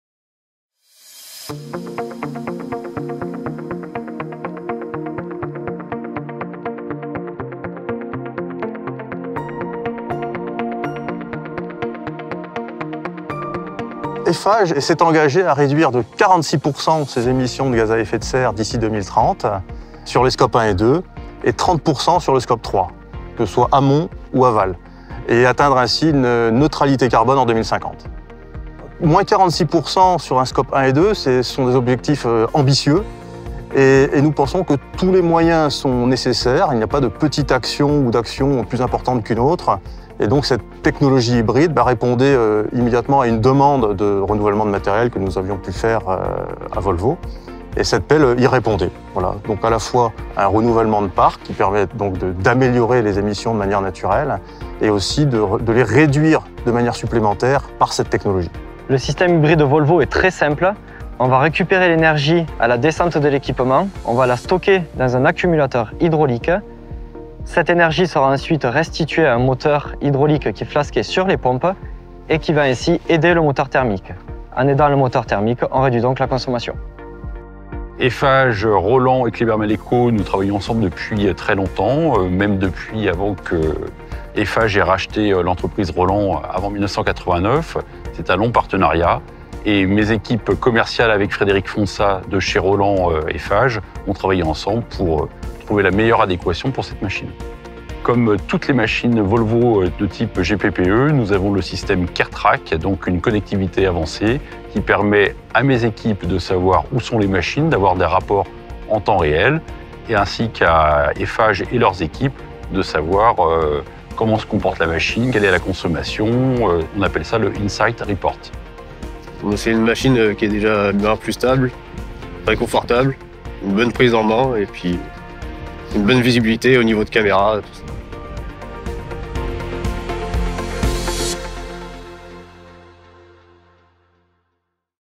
Première pelle EC250E Hybrid en France
Notre première pelle EC250E Hybrid a été livrée en France par notre concessionnaire Kleber Malecot. 🎉

Enjeux de décarbonation et réponse aux besoins de Eiffage Génie Civil, cette pelle s’inscrit dans la stratégie bas carbone de notre client.